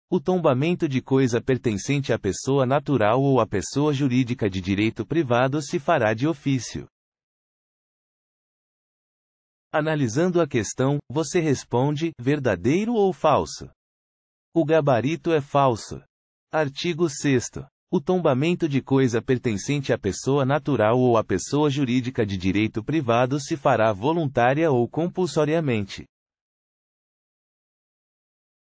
QScon Direito: [Art. 6º, DEL 25/1937] CETRO 2012 - Tabelião / Oficial de Registros - Remoção (TJ-RJ)
Art. 6º, DEL 25/1937 | CETRO 2012 | Tabelião / Oficial de Registros Remoção | (TJRJ)

O tombamento de coisa pertencente à pessoa natural ou à pessoa jurídica de. direito privado se fará de ofício.

Gabarito: Falso